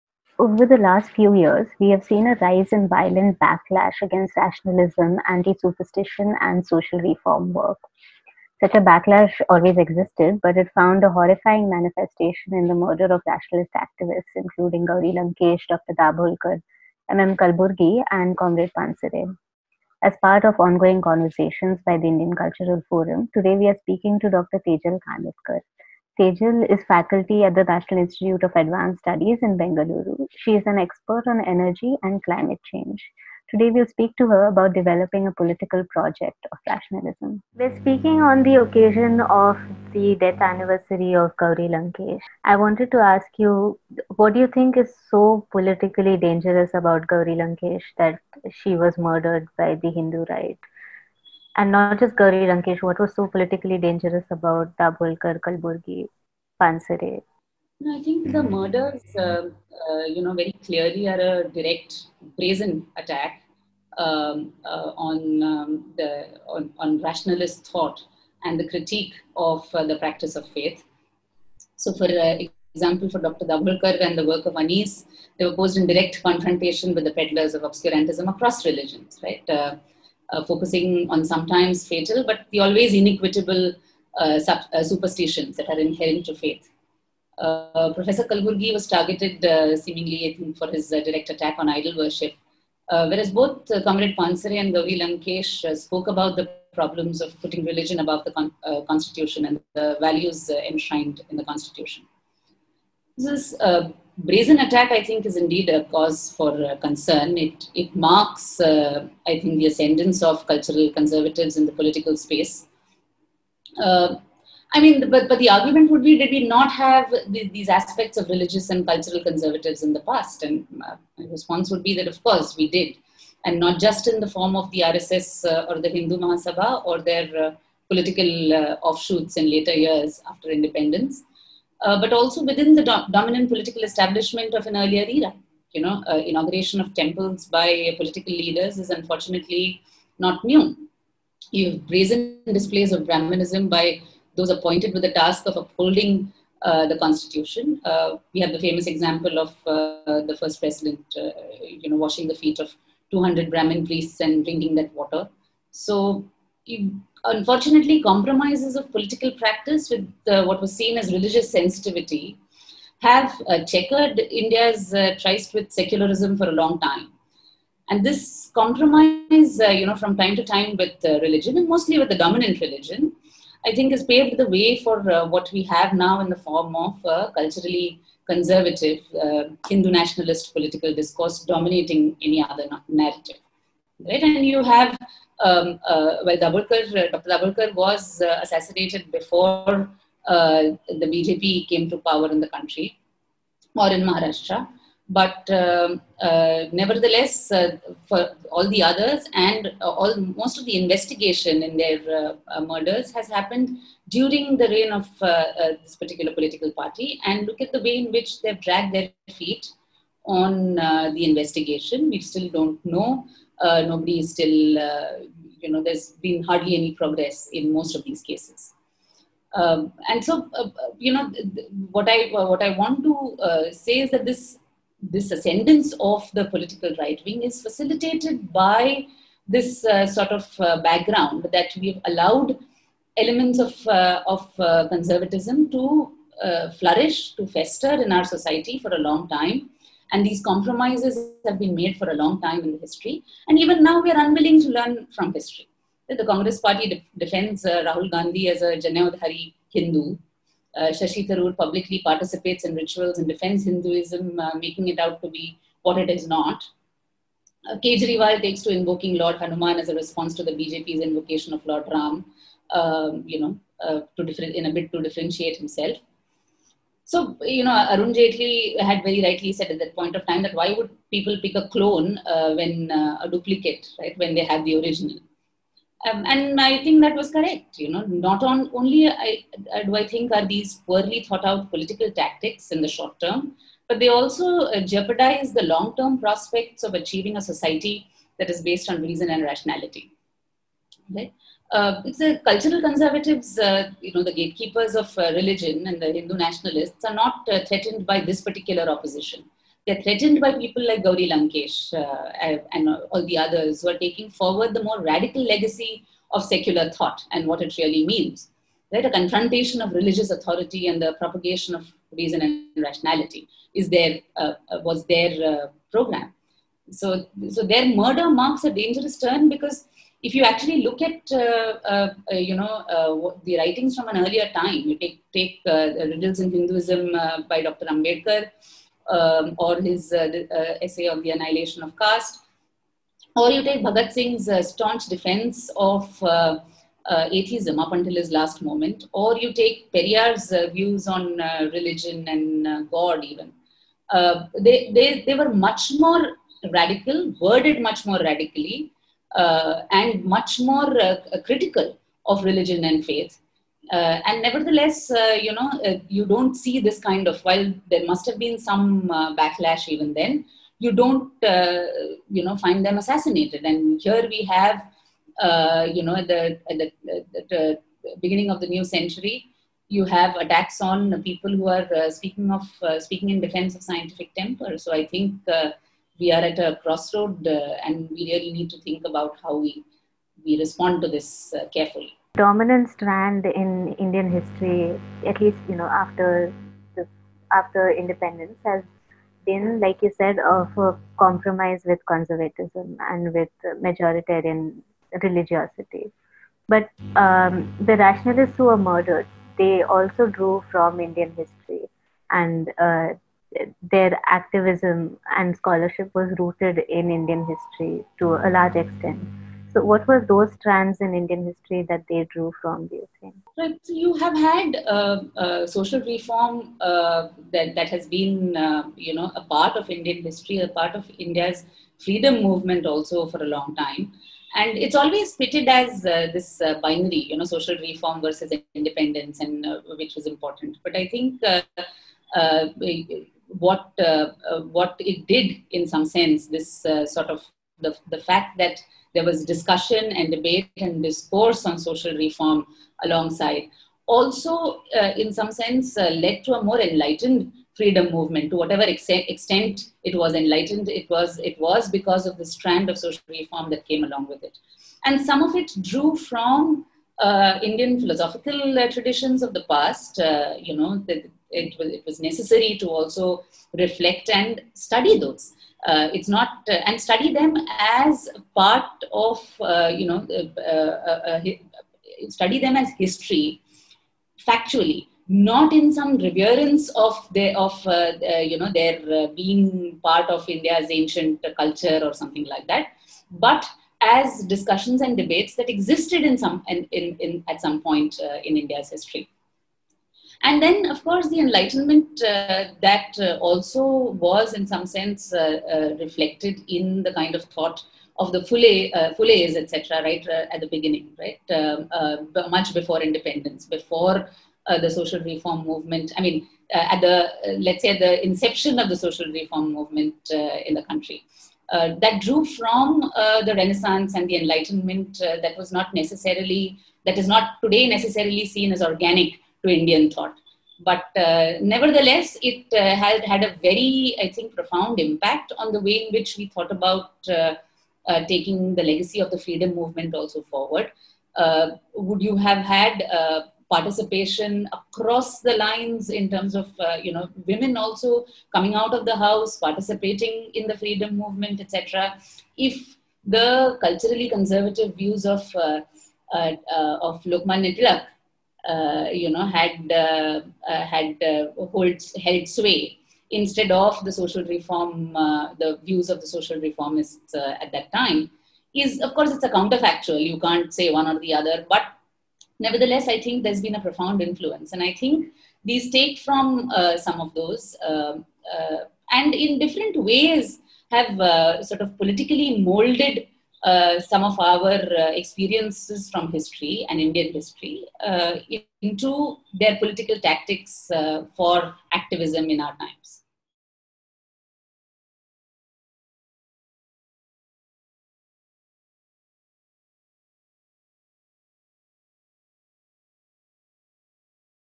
Gauri Lankesh and Rationalist Thought | Jai Vipra in Conversation with Tejal Kanitkar | Part I
Hindu Right's intolerance towards rational voices took the lives of Dabholkar, Kalburgi, Pansare and Gauri. Resistance to that intolerance continues to unite progressive voices today.

Remembering Gauri, technology policy researcher Jai Vipra speaks to Tejal Kanitkar, who is a faculty at NIAS, Bengaluru, and has been involved with the movement for rationalist thought and scientific temper.